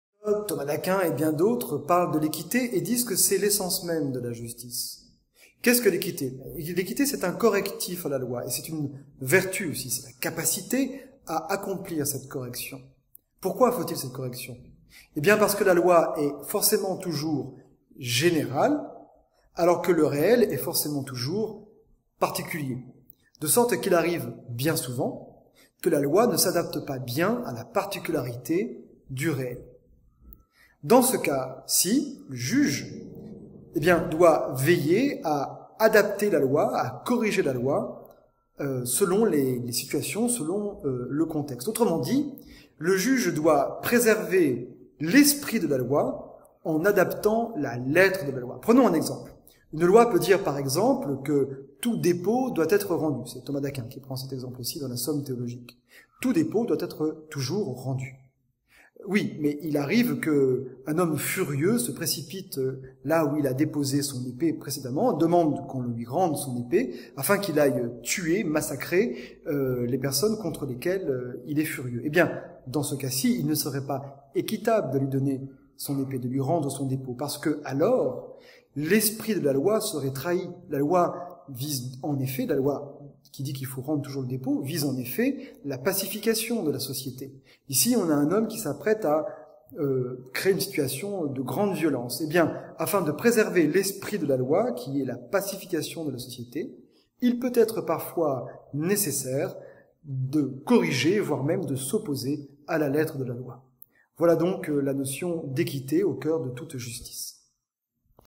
équité et justice
L'équité au cœur de toute justice : il peut-être parfois juste de ne pas suivre la loi. Référence à Thomas d'Aquin.